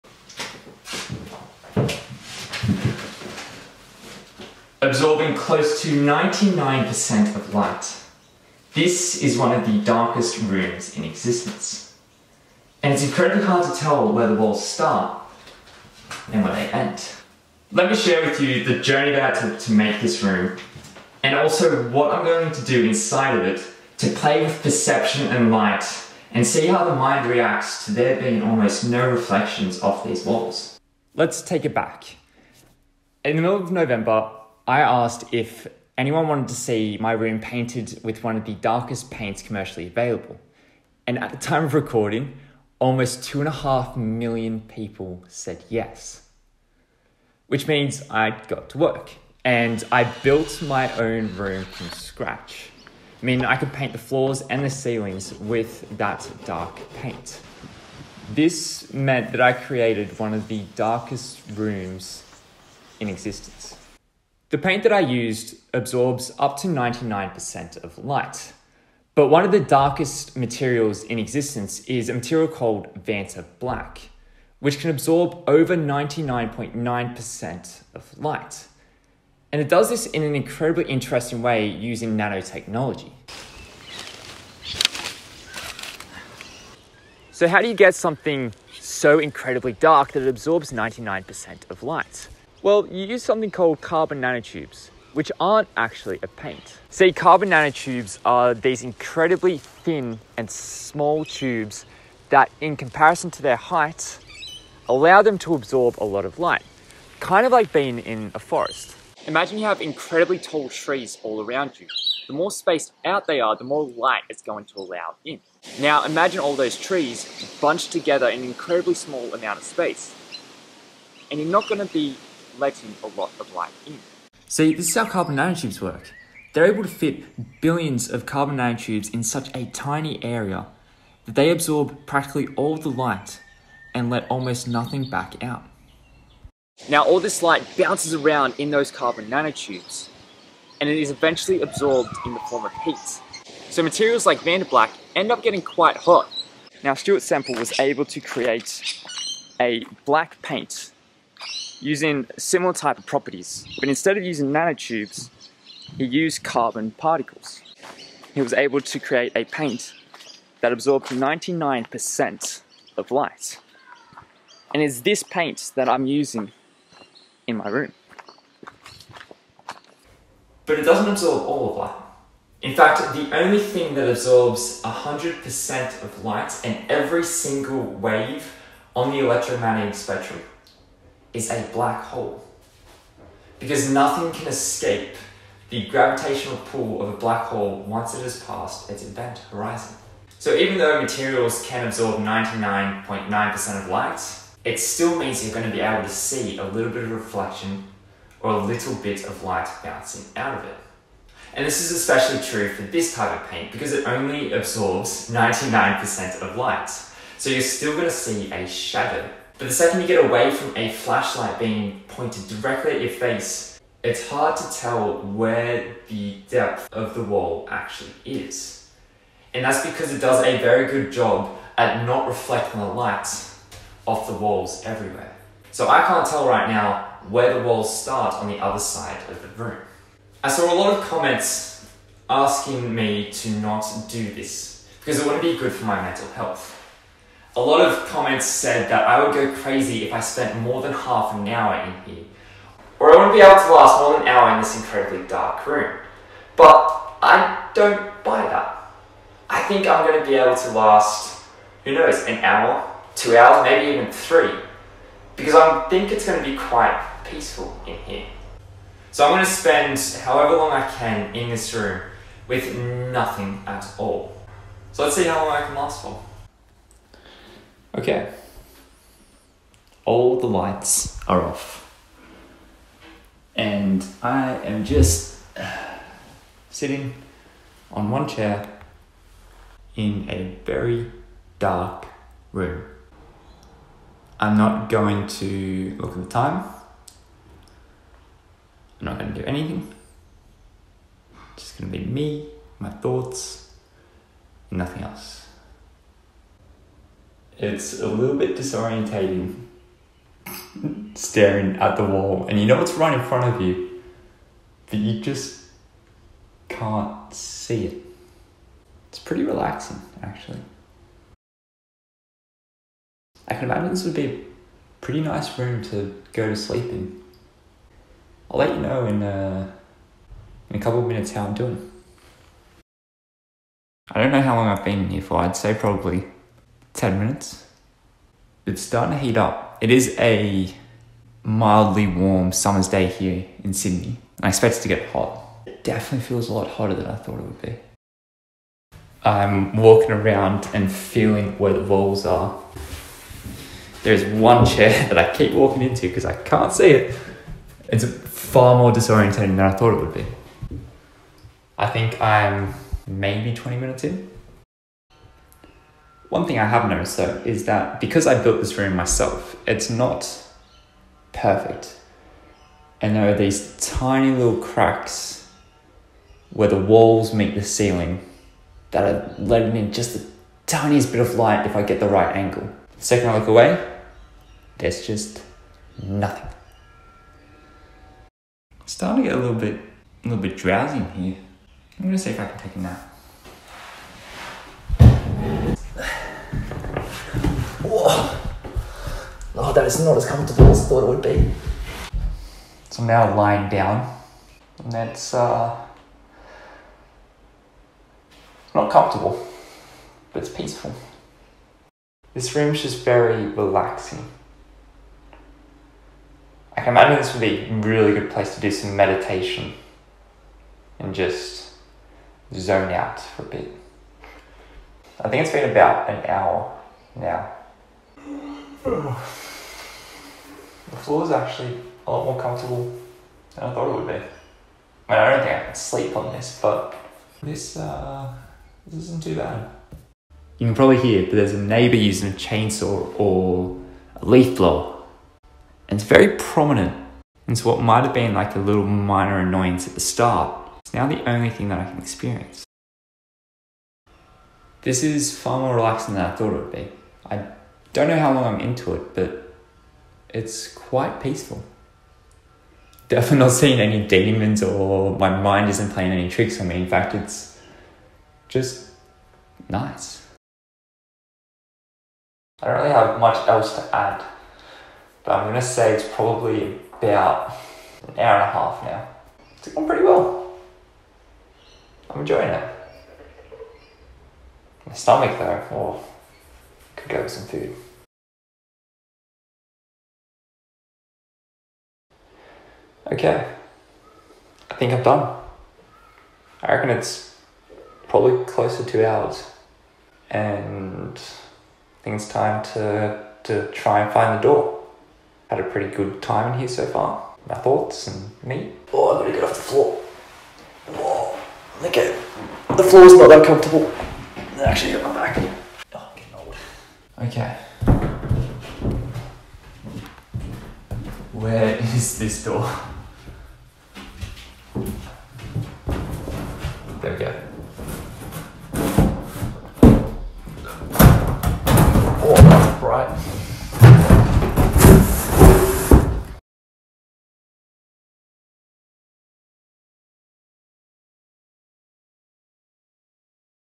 This Room Absorbs 99% of Light
I built one of the darkest rooms in the world, with Black 3.0 paint from Culture Hustle that absorbs almost 99% of light.